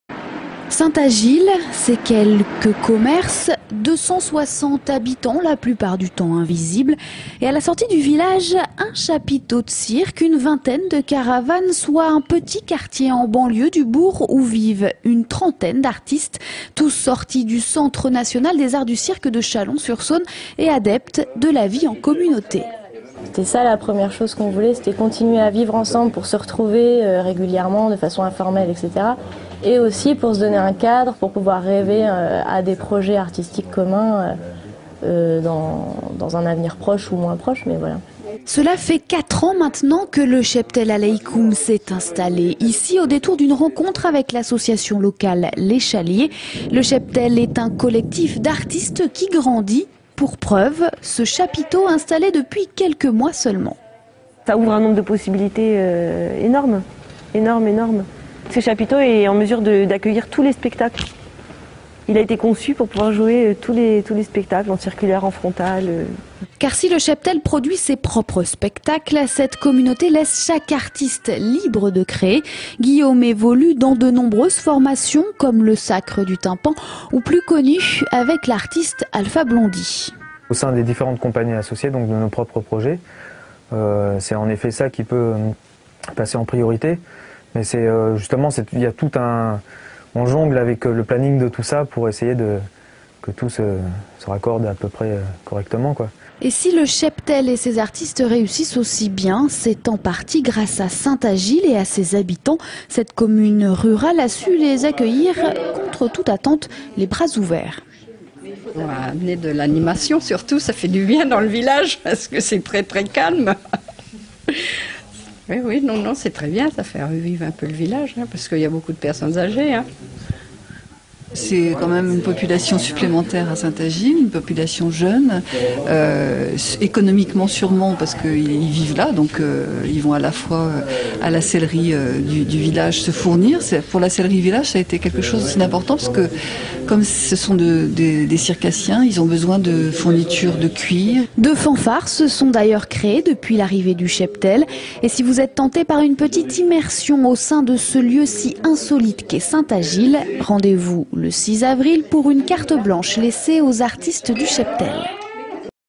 TV Tours - Le Cirque permanent à Saint Agil
04/04/2008 - TV Tours - Le Cirque permanent à Saint Agil - Tours - Une communauté d'artistes issu des arts du cirque s'est installé  à Saint Agil, dans le Loir et Cher, il y a un peu plus de 3 ans. Le "Cheptel Aleikoum" propose de nombreux spectacles tout le long de l'année, sous un chapiteau installé en permanence. La compagnie propose une spéciale "Carte Blanche", le dimanche 6 avril, à partir de 16h00, sous le chapiteau. (Pour obtenir les droits d'exploitation commerciale de cette vidéo, veuillez contacter )